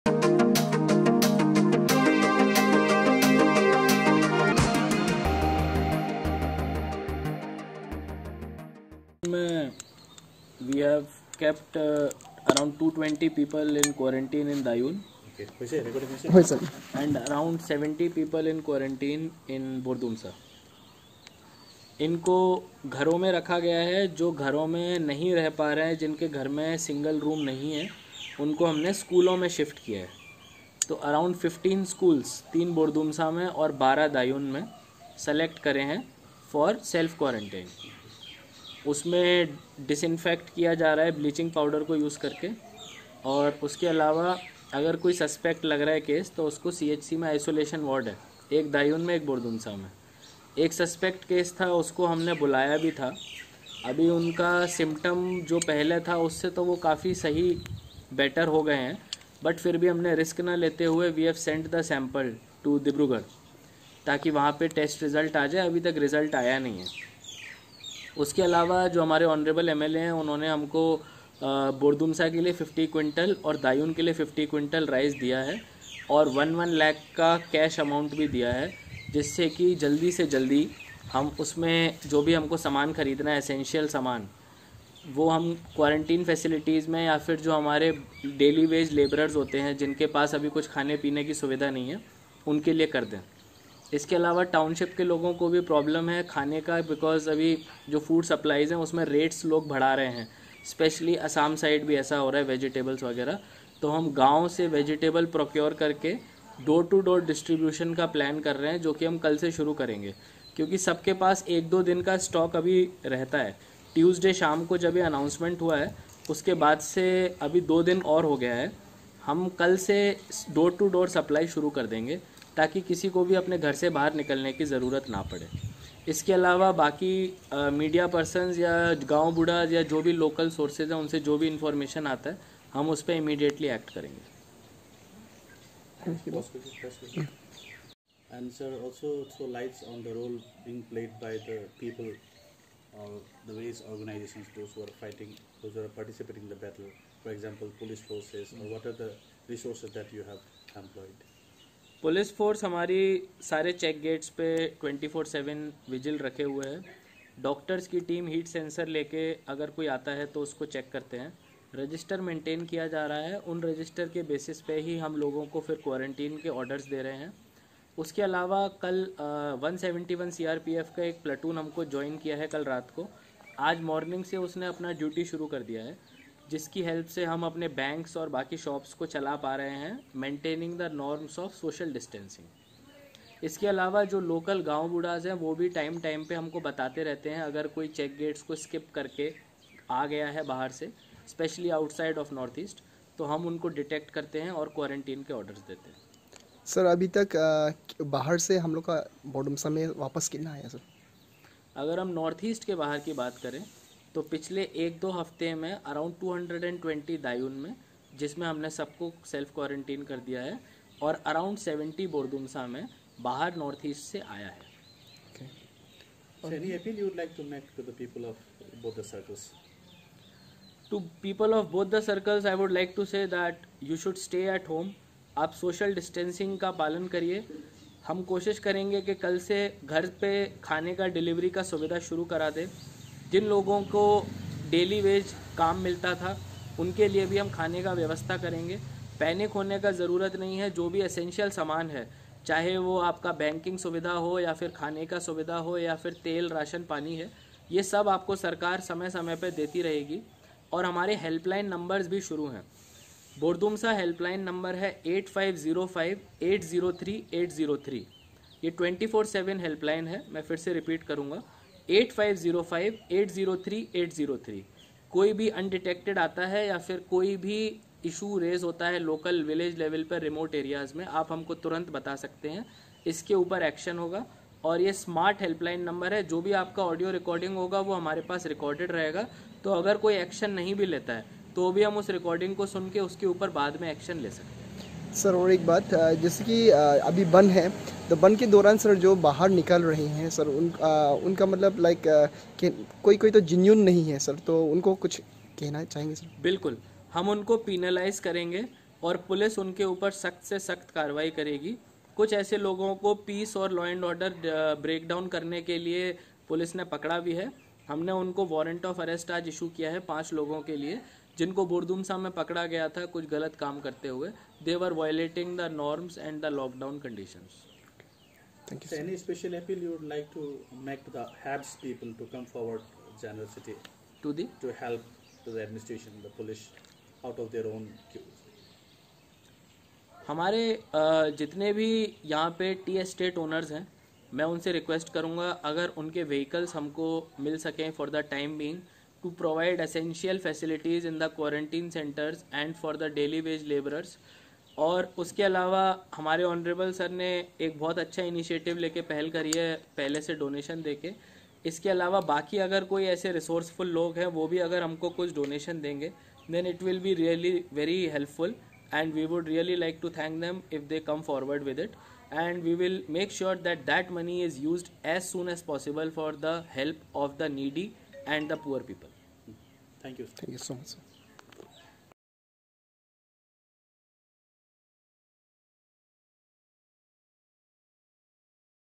More than 250 people in home quarantine in Bordumsa –Diyun Constituency
More than 250 people in home quarantine in Bordumsa –Diyun Constituency. ## 220 people in Diyun and 70 people in Bordumsa are in quarantine – ADC, Bordumsa  Dr Devansh Yadav(IAS). ## 15 schools have been designated as Isolation Center, 3 in Bordumsa and 12 in Diyun –ADC. ## Local MLA donated 100 quintals of rice and Rs 2 lakh to be used in quarantine centers.  ## Administration will also provide Home delivery of all essential items- ADC. ## Police team and Medical experts are in all the entry gates, Register is being also maintained to keep vigil on them. ## Platoon of 171 Bn CRPF has been joined the Bordumsa to support the Administration –ADC. ## Administration also providing Helpline No. for Bordumsa circle -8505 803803
Administration issues Helpline No. for Bordumsa circle -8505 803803